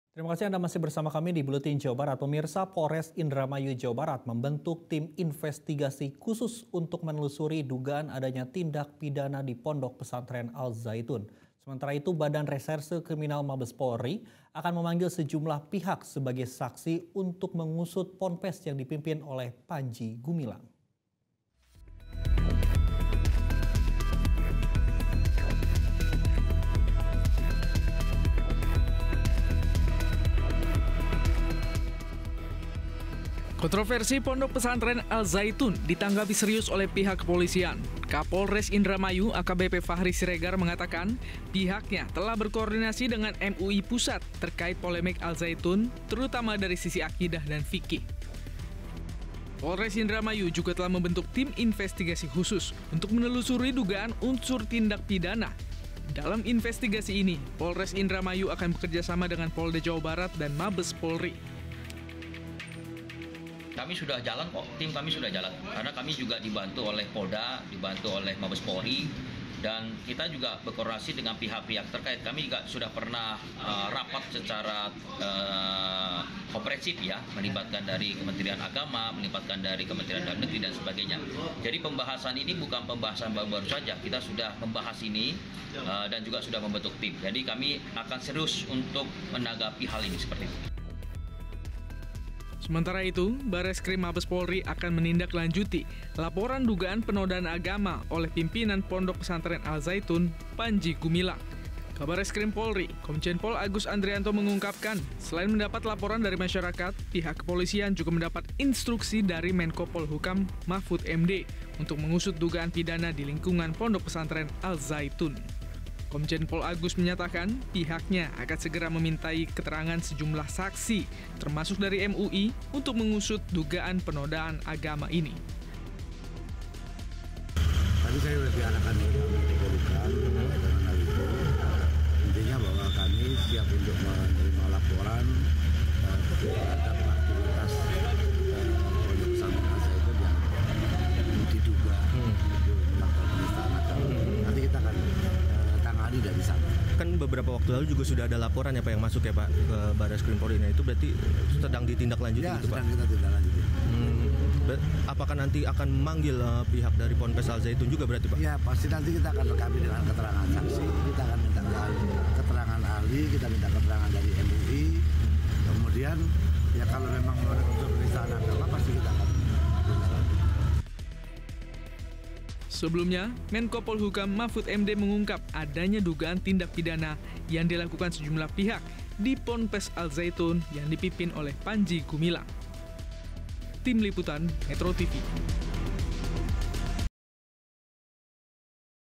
Polres Indramayu Bentuk Tim Investigasi Khusus Usut Ponpes Al-Zaytun
METRO TV - Polres Indramayu Jawa Barat membentuk tim investigasi khusus untuk menelusuri dugaan adanya tindak pidana di Pondok Pesantren Al-Zaytun. Sementara itu, Badan Reserse Kriminal Mabes Polri akan memanggil sejumlah pihak sebagai saksi untuk mengusut Ponpes yang dipimpin oleh Panji Gumilang.